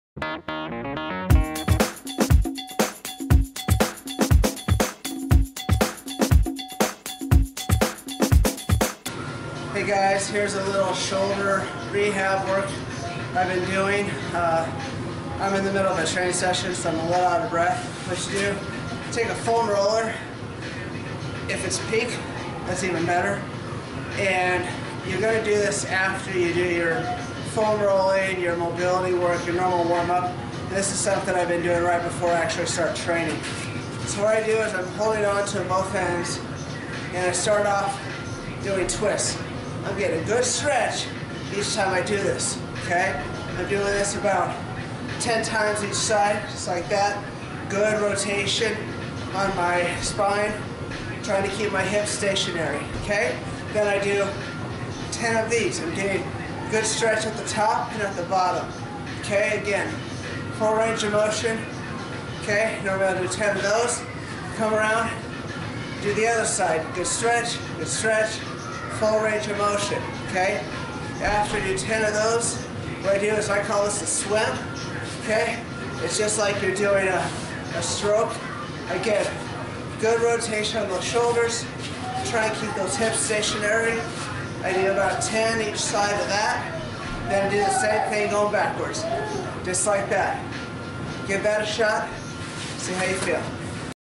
Shoulder mobility drill
This is a demonstration of a shoulder mobility drill I have been working on.  The first part of it is basically some dynamic stretching, the second part is rotation/mobility.

Ryan Irwin BS, CPT
Certified Sports Nutrition Specialist
IFPA Pro Natural Bodybuilder
Powerlifting National Champion